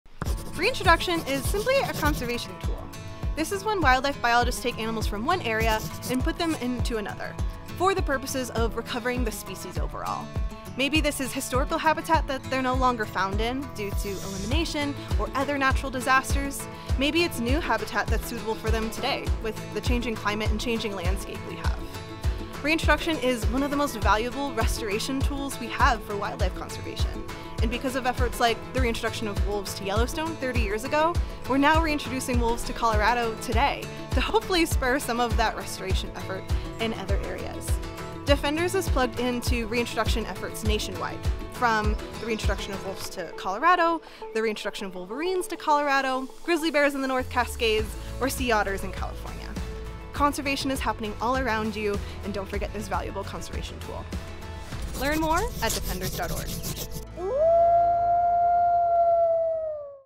Defenders Defines: Reintroduction
What does it mean exactly when a species or animal is reintroduced? What does it look like for an animal to be reintroduced back into the wild? Let us define it for you!

Transcript: Reintroduction is simply a conservation tool. This is when wildlife biologists take animals from one area and put them into another for the purposes of recovering the species overall. Maybe this is historical habitat that they're no longer found in due to elimination or other natural disasters. Maybe it's new habitat that's suitable for them today with the changing climate and changing landscape we have. Reintroduction is one of the most
valuable restoration tools we have for wildlife conservation. And because of efforts like the reintroduction of wolves to Yellowstone 30 years ago, we're now reintroducing wolves to Colorado today to hopefully spur some of that restoration effort in other areas. Defenders is plugged into reintroduction
efforts nationwide from the reintroduction of wolves to Colorado, the reintroduction of wolverines to Colorado, grizzly bears in the North Cascades,
or sea otters in California. Conservation is happening all around you, and don't forget this valuable conservation tool.